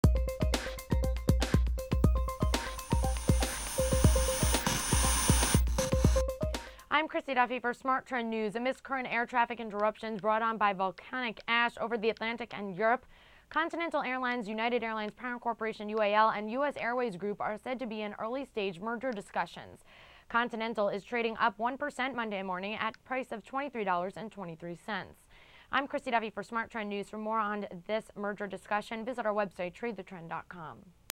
News Update: Volcanic Ash Brings Out Potential Airline Merger Talks (CAL,UAUA,LCC)
Amidst current air traffic interruptions brought on by volcanic ash over the Atlantic and Europe, Continental Airlines, Inc. (NYSE:CAL), United Airlines parent corporation UAL (NASDAQ:UAUA), and US Airways Group, Inc. (NYSE:LCC) are said to be in early-stage merger discussions.

Continental Airlines is up 1.09% at $23.23.